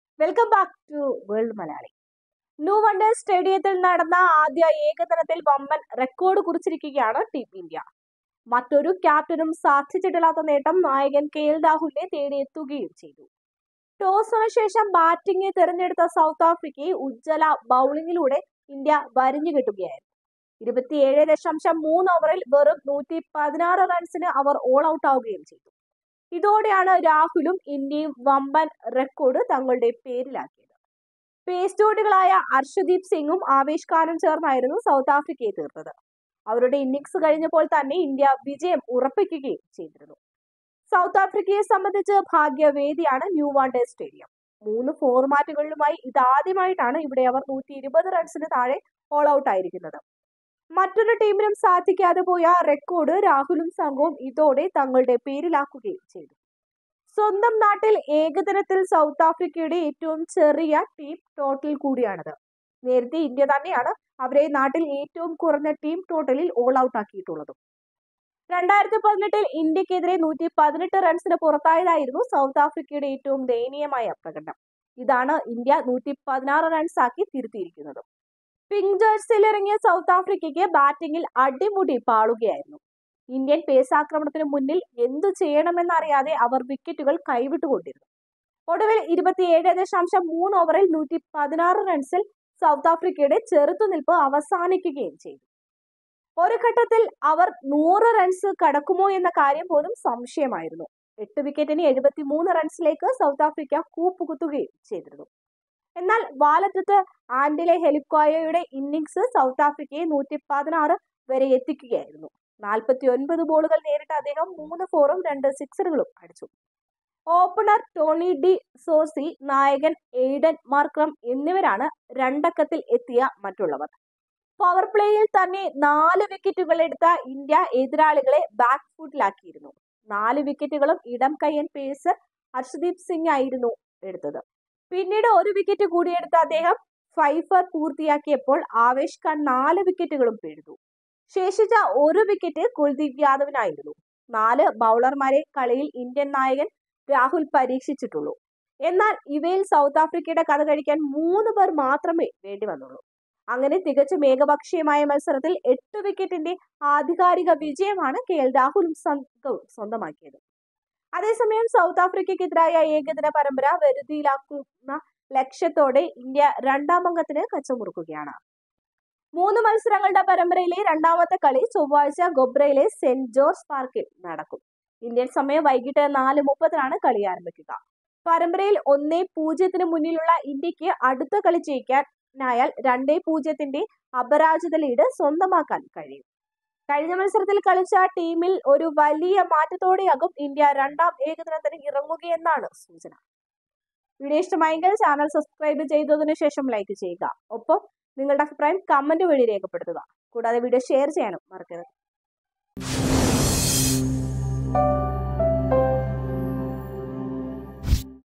ദക്ഷിണാഫ്രിക്കയെ തകർത്ത് കെഎൽ രാഹുലും സംഘവും നേടിയെടുത്തത് മറ്റാർക്കും സാധ്യമാകാത്ത കിടിലൻ റെക്കോർഡ്
ദക്ഷിണാഫ്രിക്ക യെ തകർത്ത് കെ എൽ രാഹുലും സംഘവും നേടിയെടുത്തത് മറ്റാർക്കും സാധ്യമാകാത്ത കിടിലൻ റെക്കോർഡ്..